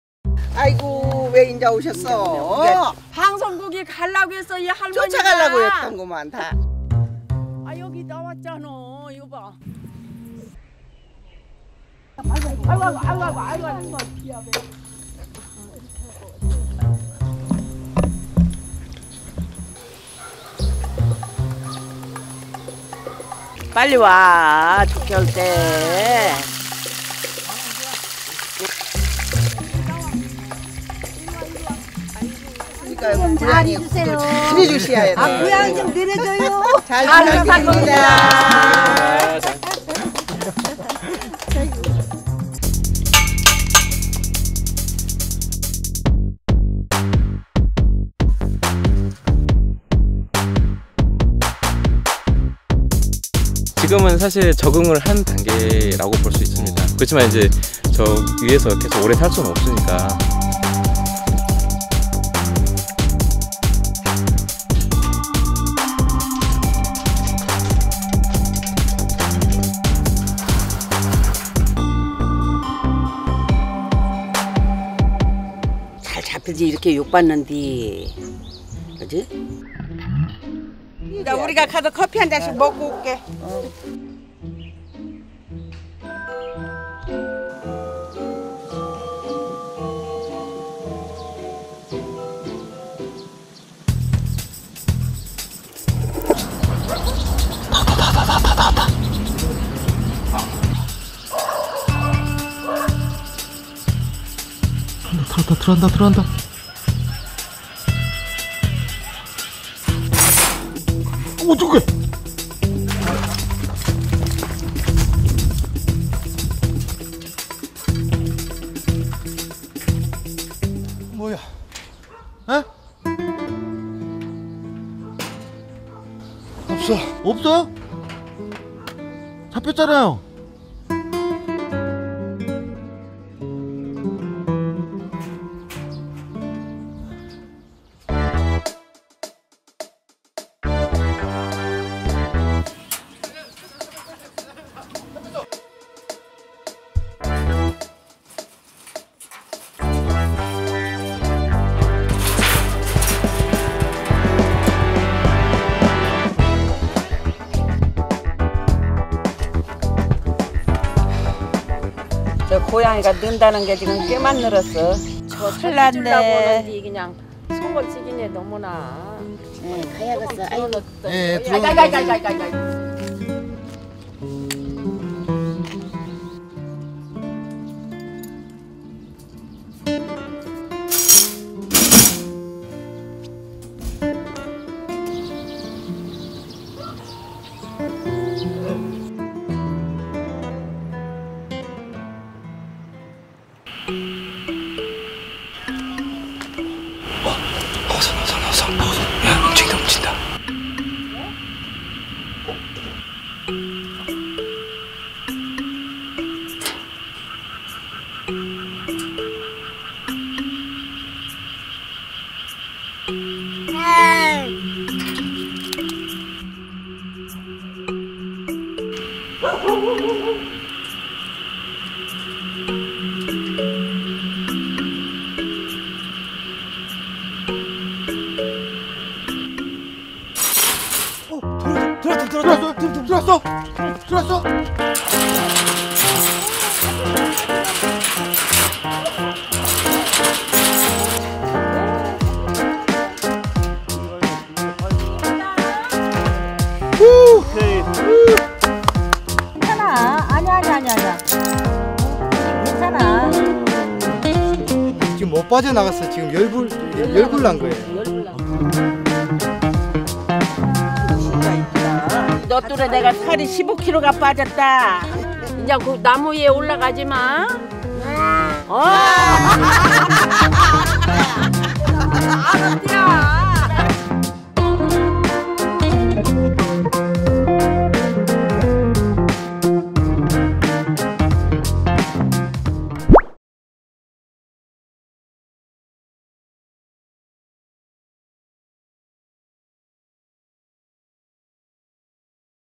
Old Ladies Takes The Cat Up In The Down In A Shocking Way (Part 2) | Kritter Klub
Old ladies in the town all gathered to rescue the cat stranded on top of the tree. Plucking out their old wisdom, the grandmothers come up with a brilliant way to get the cat down, which is..?